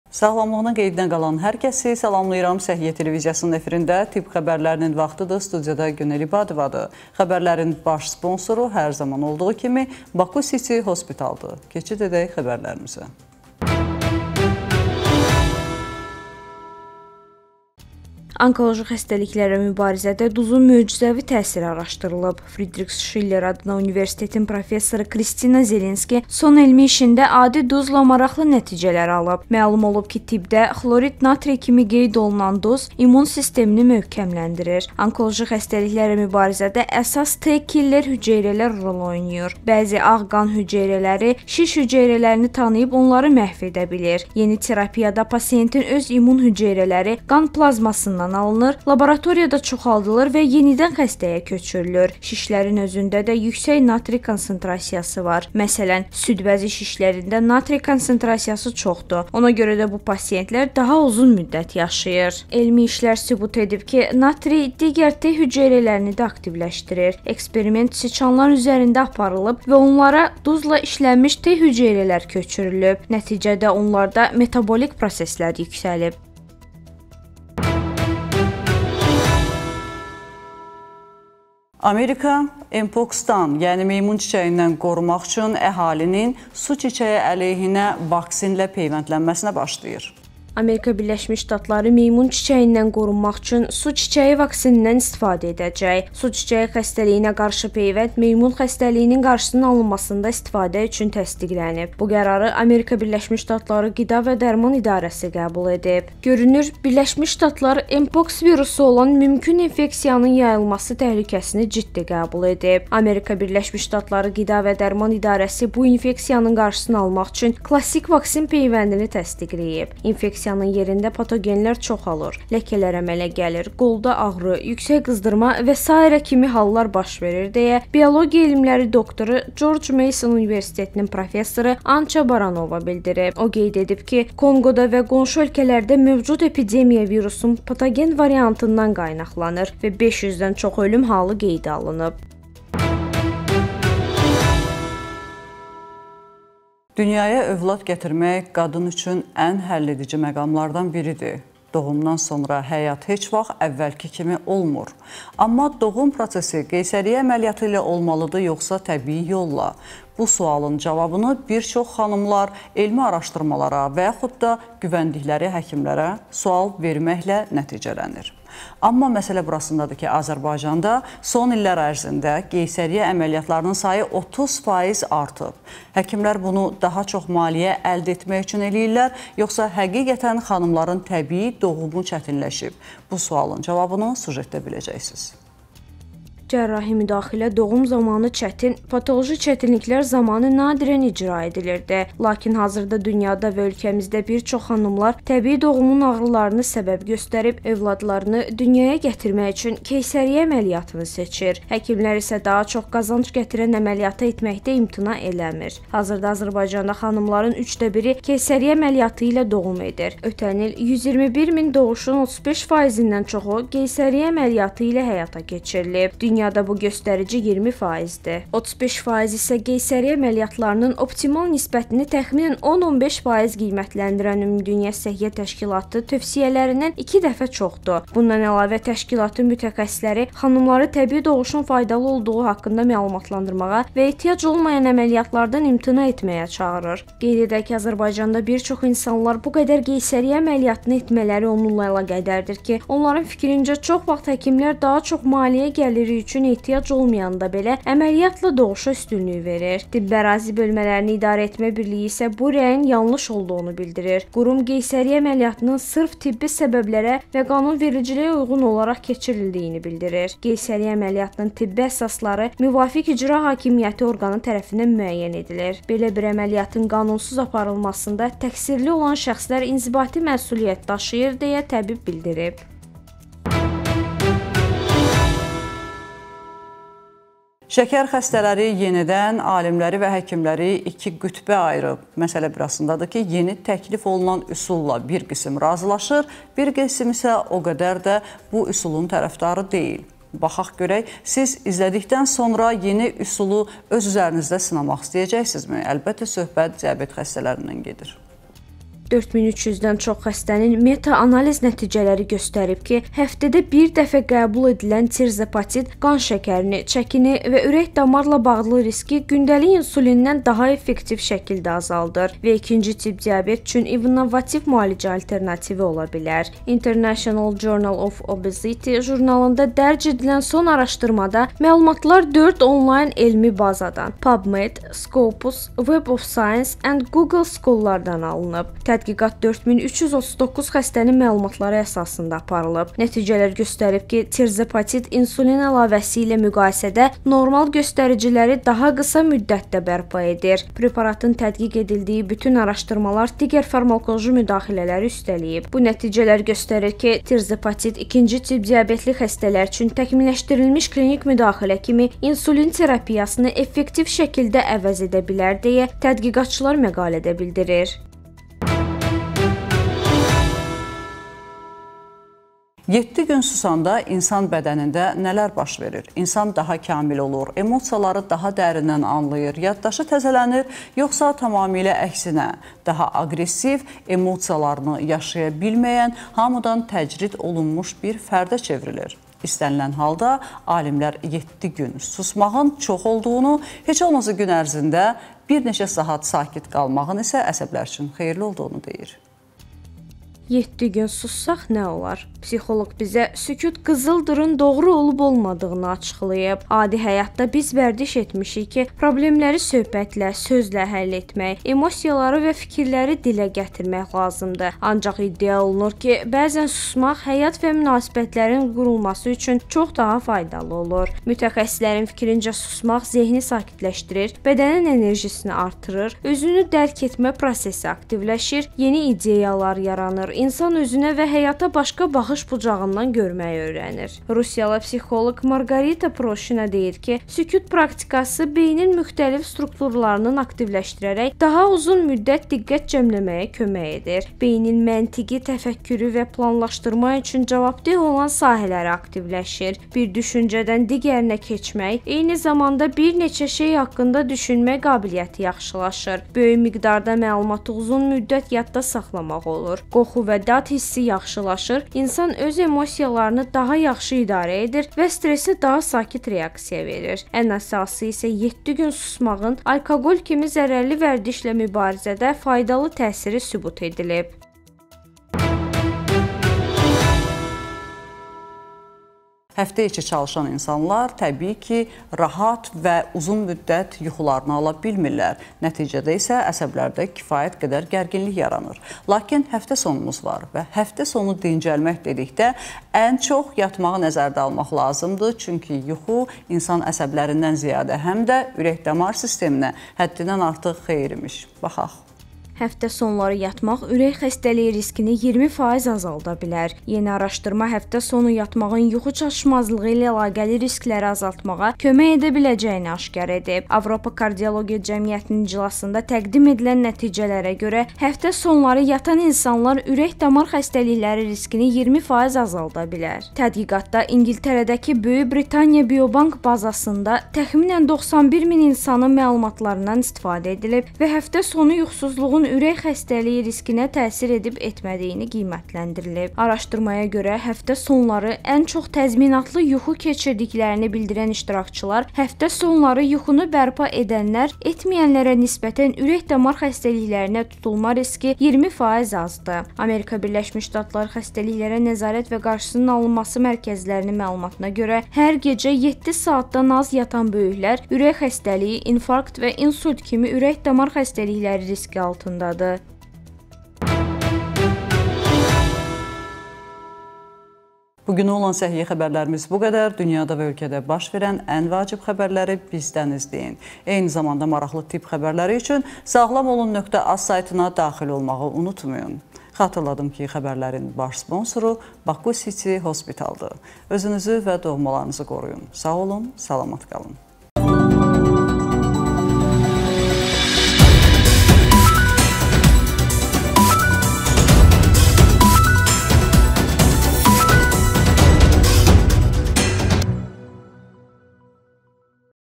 ŞƏKƏR XƏSTƏLİYİNİN YENİ MÜALİCƏSİ
00:00

00:17 XƏRÇƏNG ƏLEYHİNƏ YENİ ÜSUL
ALİMLƏRİN YENİ ARAŞDIRMASI

01:21 MEYMUN ÇİÇƏYİ ƏLEYHİNƏ SU ÇİÇƏYİ PEYVƏNDİ İSTİFADƏ OLUNACAQ
QİDA VƏ DƏRMAN İDARƏSİ QƏRAR QƏBUL EDİB

02:26 KEYSƏRİYYƏ ƏMƏLİYYATI ÜÇ DƏFƏ ÇOXDUR
TƏBİİ DOĞUM, YOXSA CƏRRAHİ MÜDAXİLƏ?!

04:37 ŞƏKƏR XƏSTƏLİYİNİN YENİ MÜALİCƏSİ
“JOURNAL OF OBESITY” YAZIR

06:02 7 GÜN DANIŞMASAQ NƏ OLAR?
SUSMAQ – HƏQİQƏTƏN QIZILDIR? PSİXOLOQ AÇIQLAYIR

07:49 HƏFTƏSONLARI YATMAQ ÜRƏK XƏSTƏLİYİ RİSKİNİ AZALDIR
YENİ ARAŞDIRMA